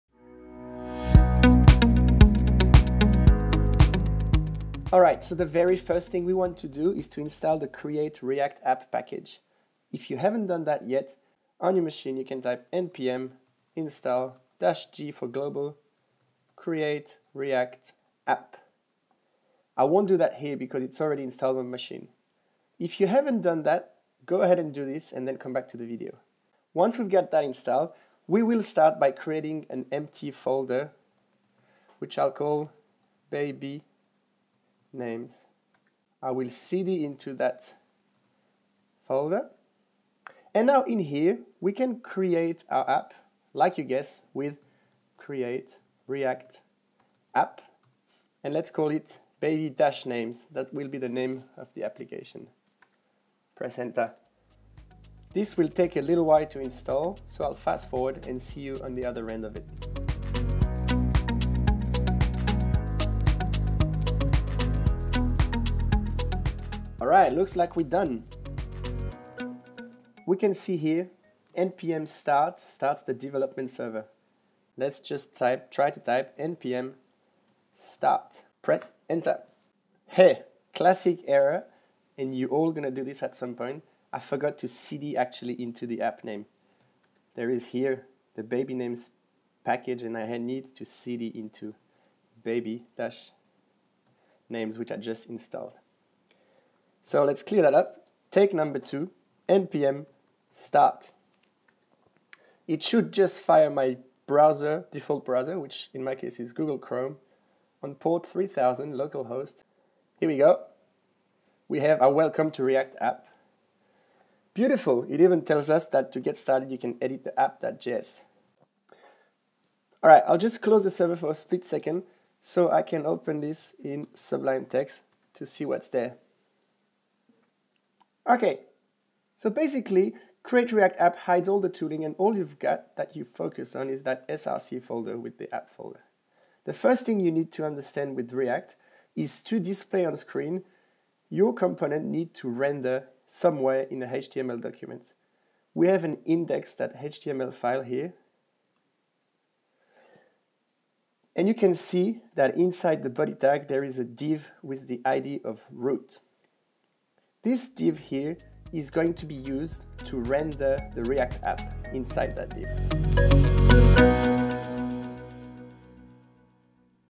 ReactJS Beginner Series: #2 - Installing Create React App
**2020 UPDATE**: A brand new version of this app, built with React hooks and the Context API, is available for free on egghead.io

In this video, we will create a brand new application project using Facebook's Create React App.

Series Content

#1 - Series Intro
#2 - Installing Create React App

We'll build the app from the ground up, and learn how to implement the following:

- display a list of names from a data array
- filter names via user input field
- clear the search via button click
- short list clicked names as favourites
- delete shortlisted names on click

We'll also cover the following aspects:

- React components as ES6 classes
- Stateless, functional component syntax
- ES6 module  import / export
- deploying the app for FREE on surge.sh

Links